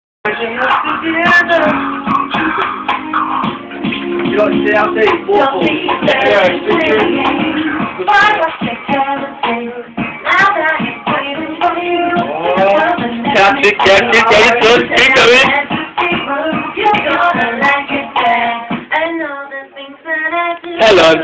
New Years Party at Staceys 2
New years party at staceys well afta everyone had left...Half 6 in the mornin dancin 2 cheesy choon wrecked!